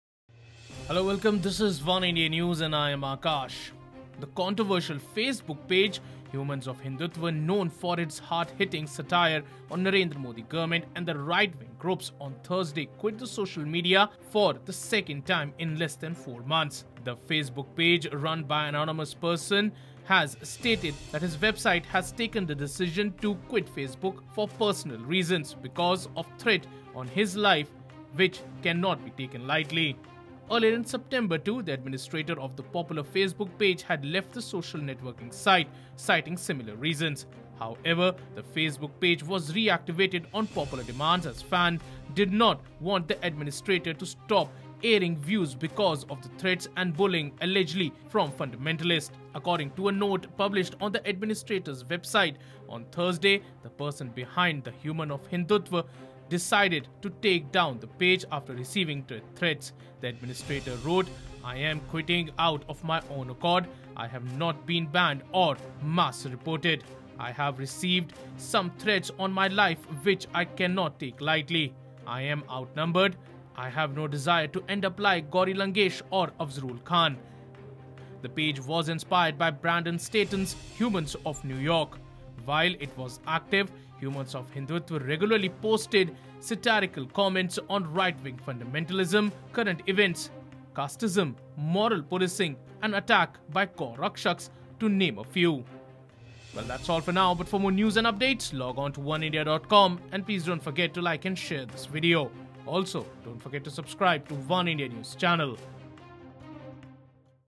Controversial Facebook page, Humans of Hindutva quits social media | Oneindia News
The controversial Facebook page, Humans of Hindutva, known for its hard-hitting satire on the Narendra Modi government and right-wing groups, on Thursday quit the social networking site for the second time in less than four months. The page had in September quit social media, but was activated once again after its fan asked them to, but this the administrator says that he is getting personal threats and does not want his end to be like Gauri Lankesh. While some on the social media called this incident as an attack on free speech there are few who cheered the closure of the page. Watch this video to find out more.